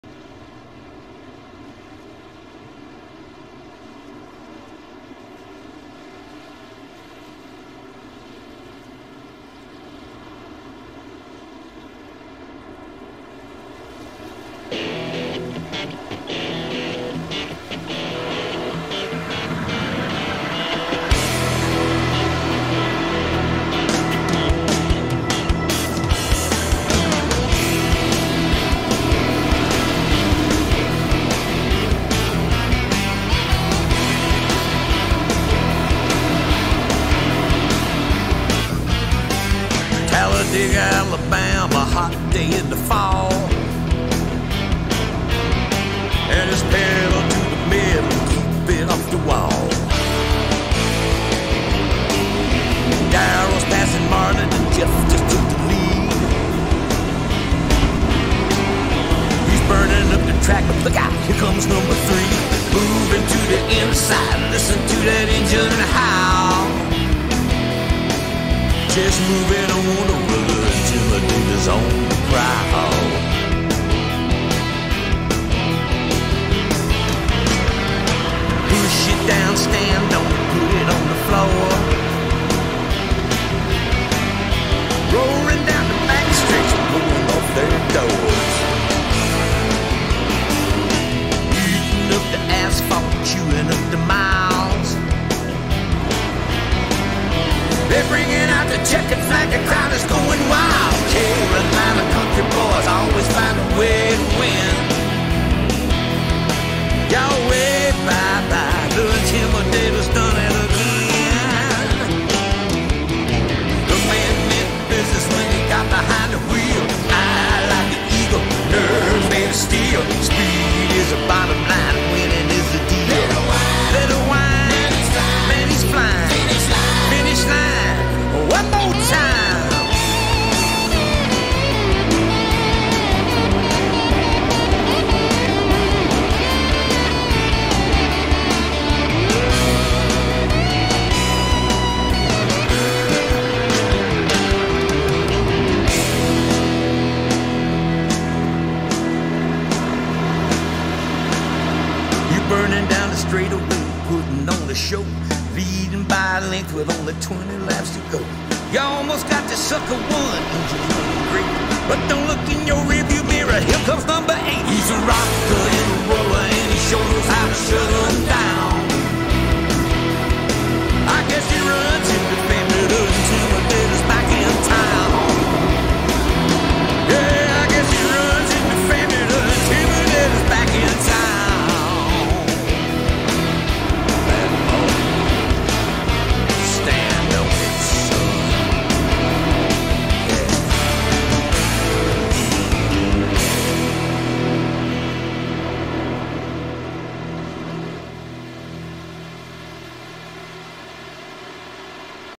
The Simpit Oval League Opener
COTA in the NASCAR Trucks for The Simpit Oval League Opener. Enjoy!